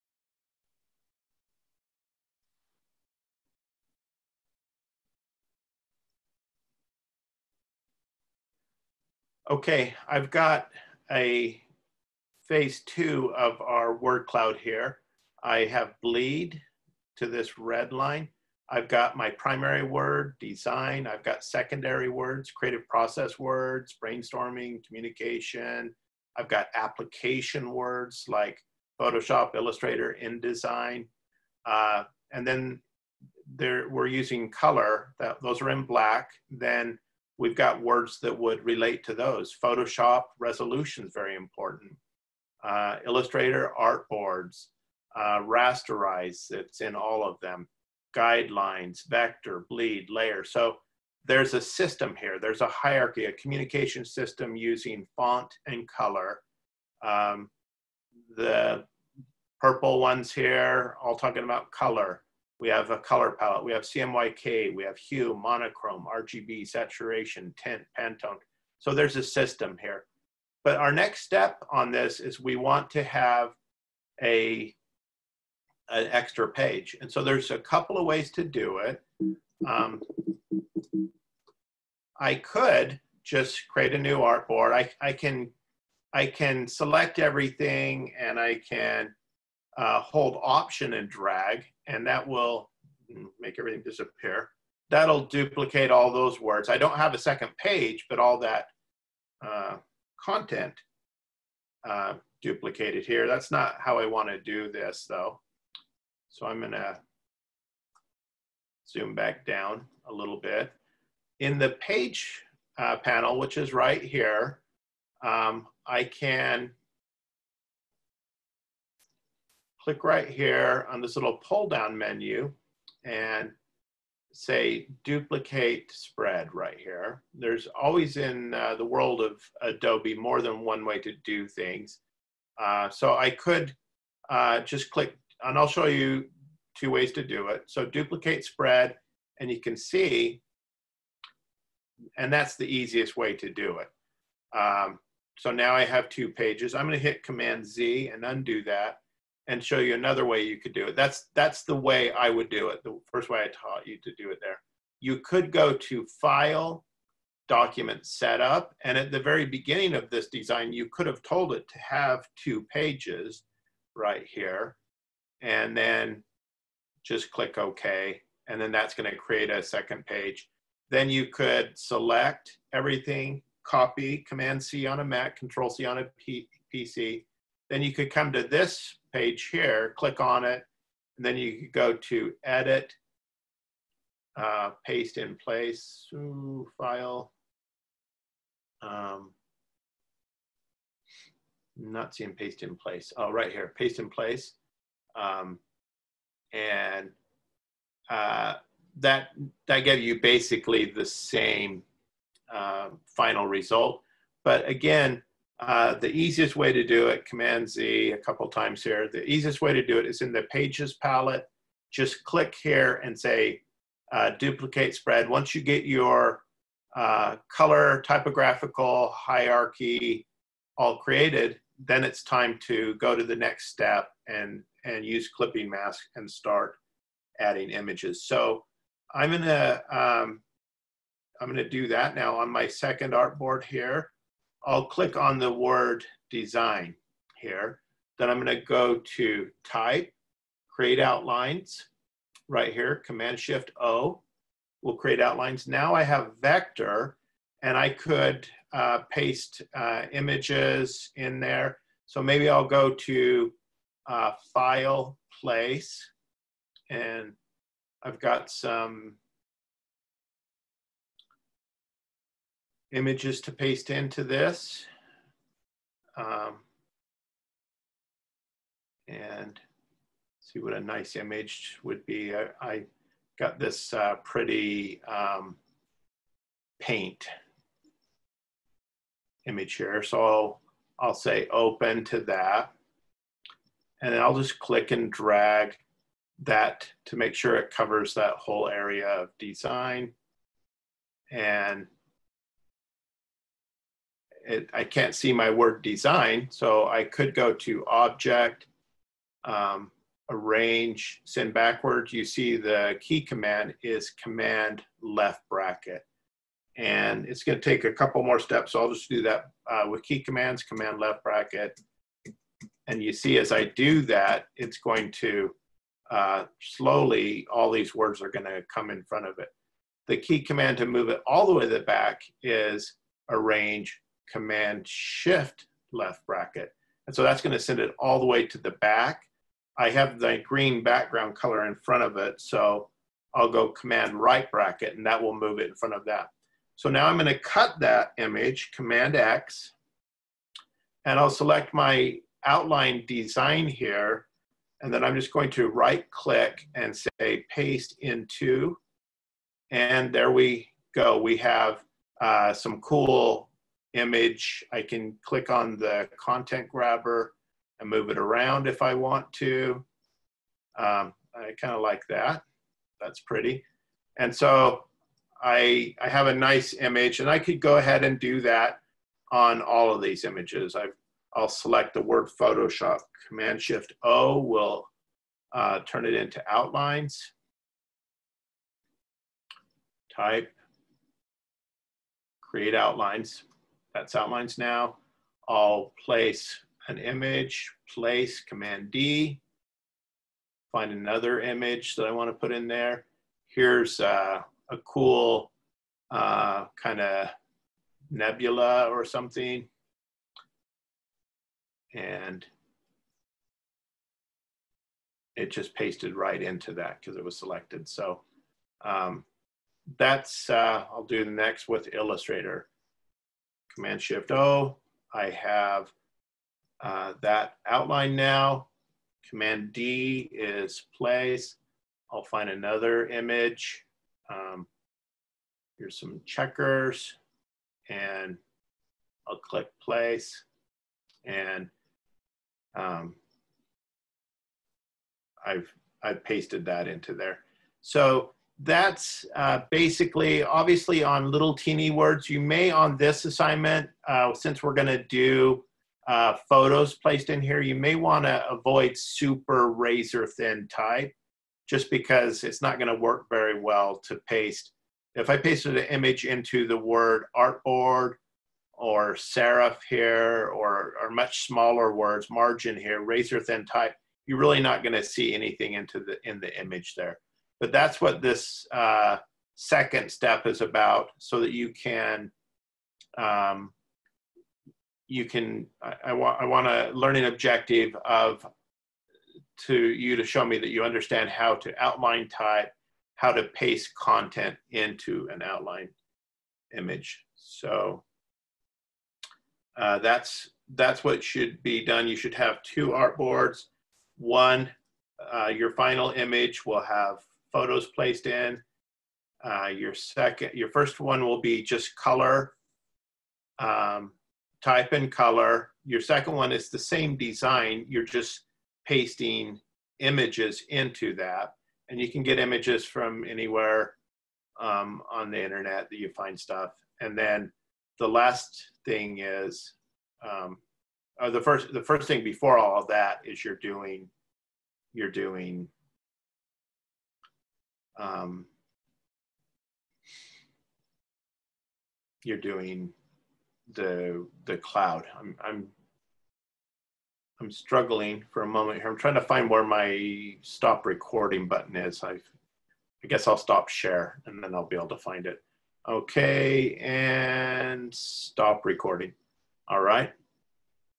InDesign-Duplicate Pages-Clipping Mask Type
InDesign: How to duplicate pages. How to convert type to a vector outline. How to paste images into typography.